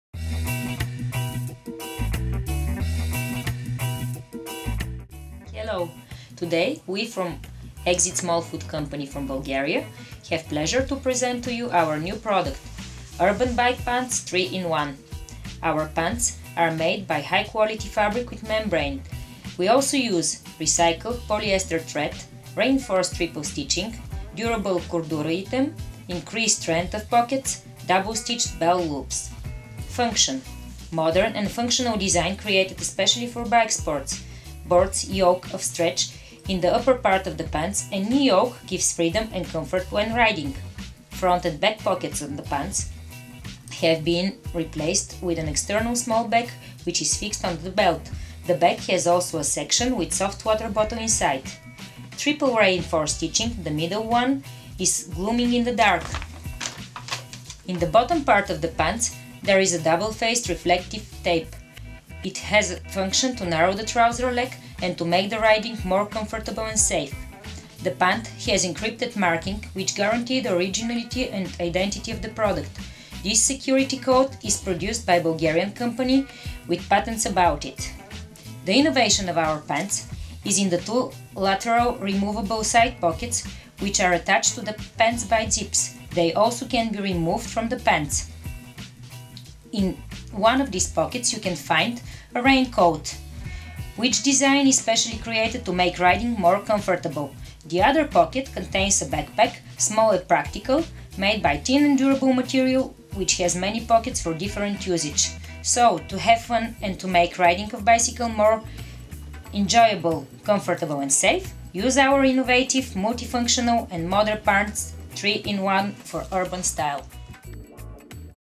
Small Foot Bike Pants 3 in 1
Short video about our new product - bike pants 3 in 1.